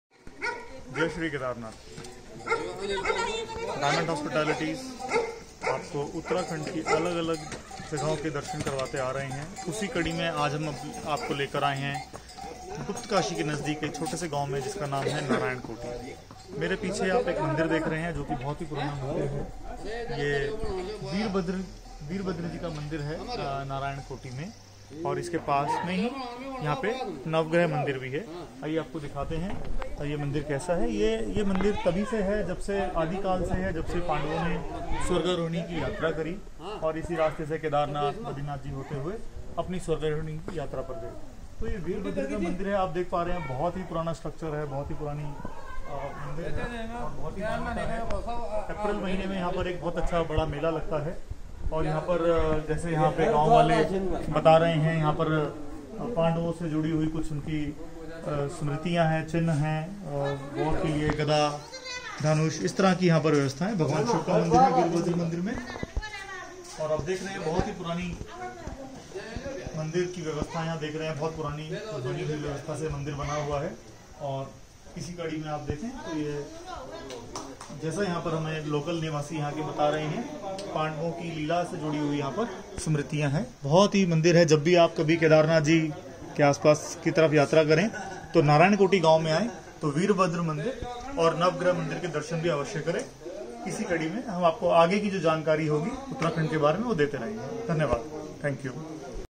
Char Dham Yatra. Narayankoti Temple Guptkashi enroute to Shri Kedarnath Yatra. Beautiful Uttarakhand
Naryankoti Temple, cluster of small historic temples in nearby surroundings. Laxmi Narayan Temple, Virbhadra Temple, Navagrah Temple and many more. Related with marriage of Lord Shiva and Parvati.
Some locals also relate these temples with journey of Pandavas after Mahabharata. Century old temples a must visit place during your stay at Guptkashi or else enroute to visit to Shri Kedarnath temple.

For details please write at